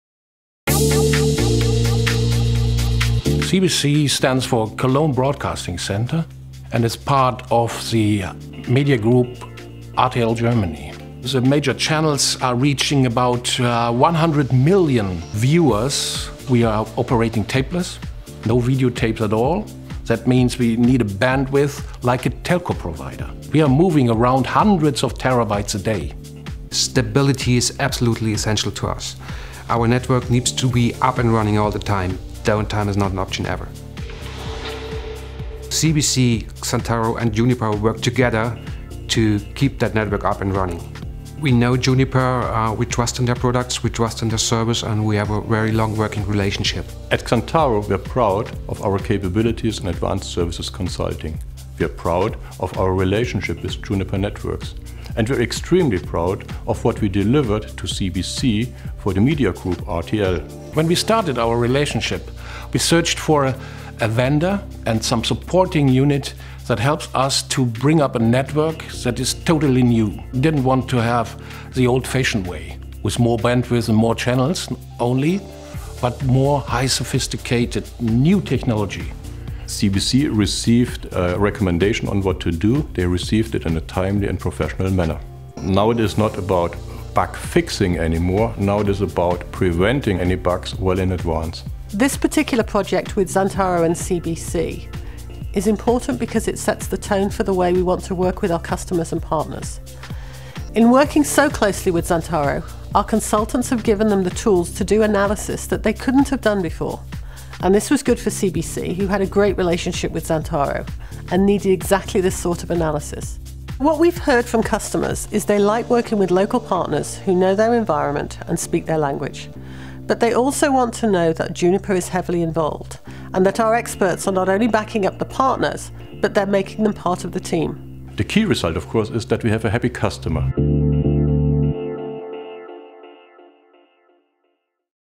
Evolving Broadcaster CBC's Network
CBC is a major television broadcaster in Germany. It needs to transport huge High Definition video files between its major centers in Cologne, Munich and Berlin. Evolving its Juniper-based network to meet these needs required detailed analysis and recommendations. CBC relied on Juniper partner Xantaro to do this analysis. Xantaro's expert consultants worked closely with Juniper Networks consultants, who gave them access to tools and methodologies generally only available to internal Juniper staff. The resulting network upgrade satisfied CBC's needs for a smooth evolution of the network.